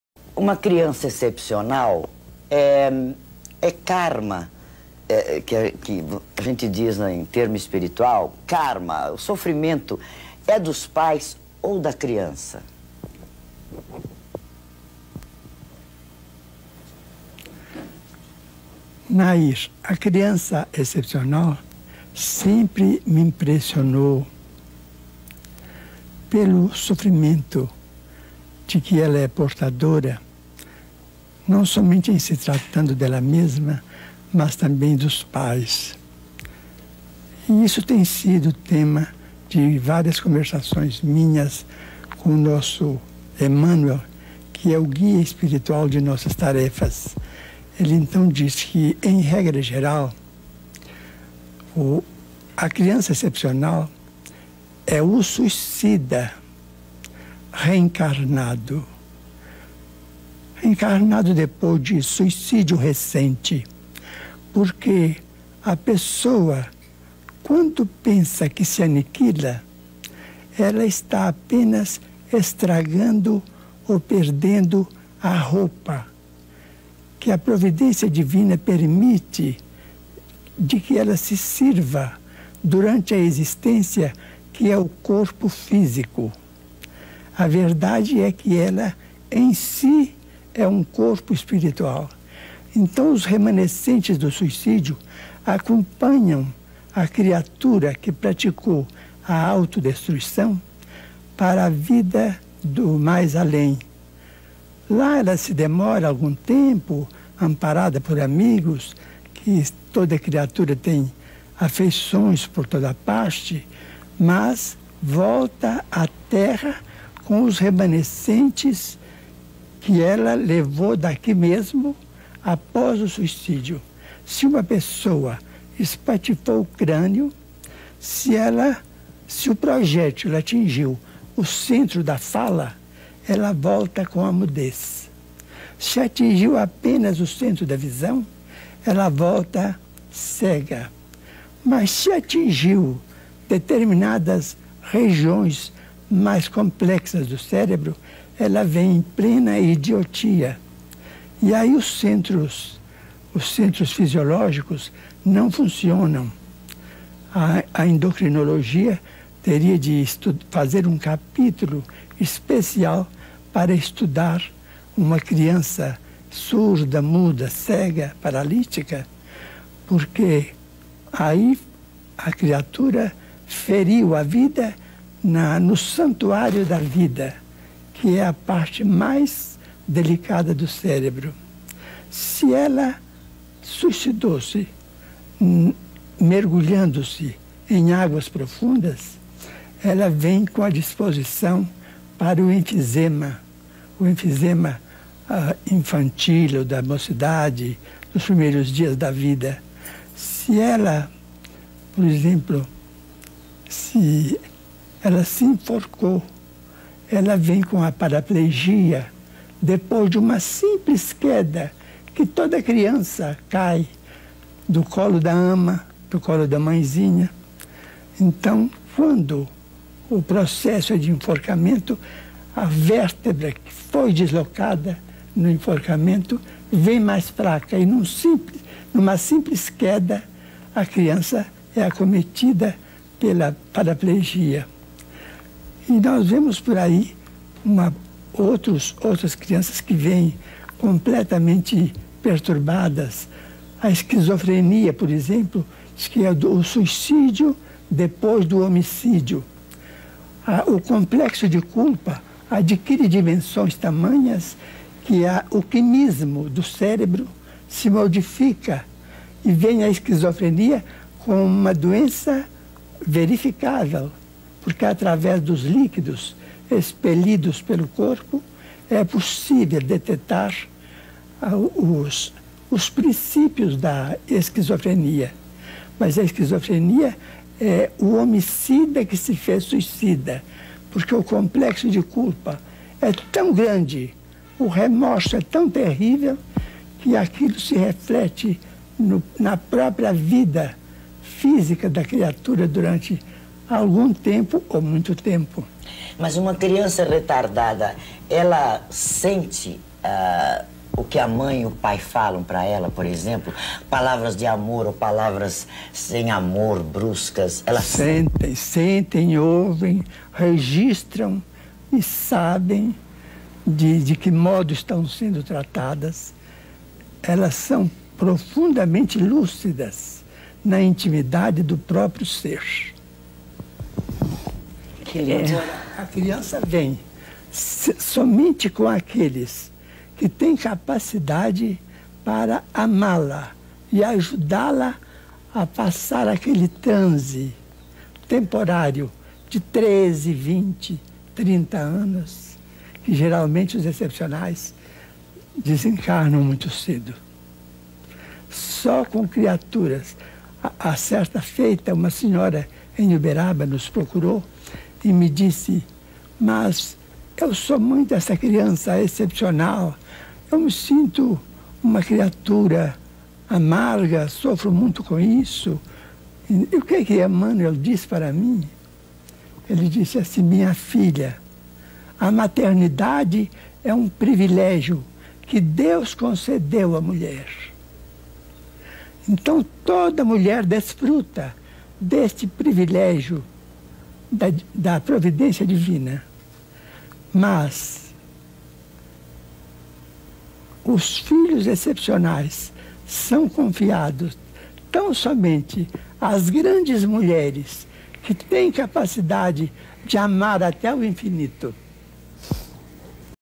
Chico Xavier sui bambini speciali
Chico Xavier parla degli effetti del suicidio sulla reincarnazione successiva